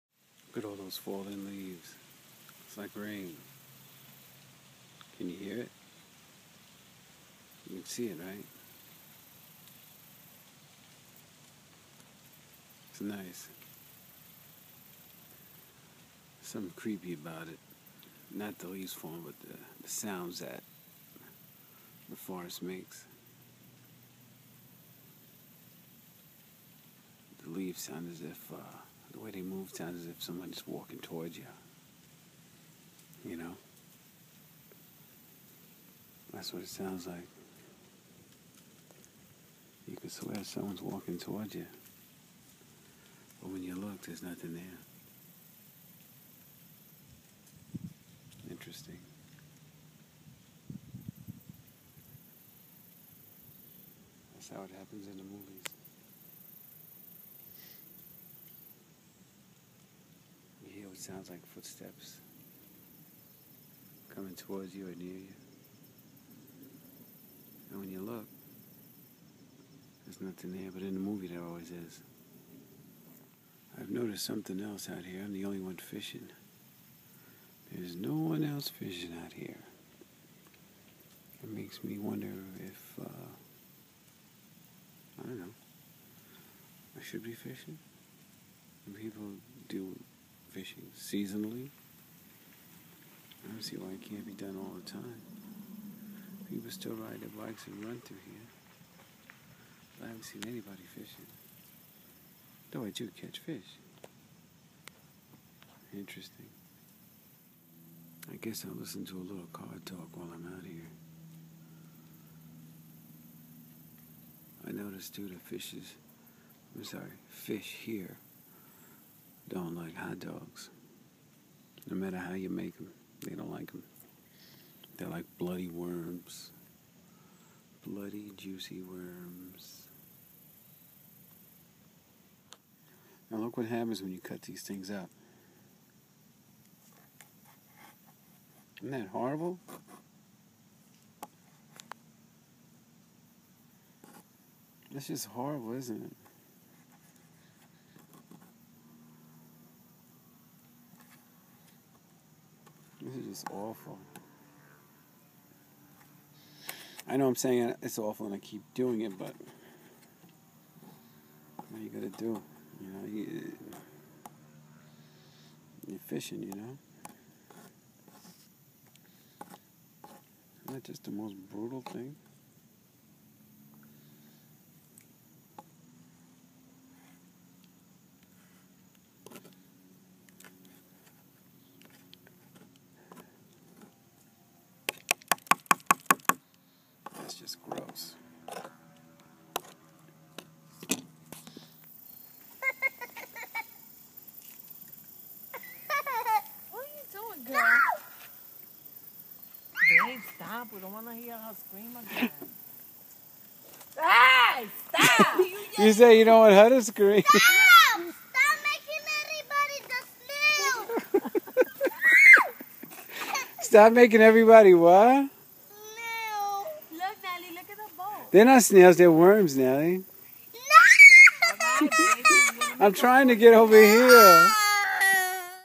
Fishing and Cutting Worms
Remarking on how spooky it is while in the forest and no one's around.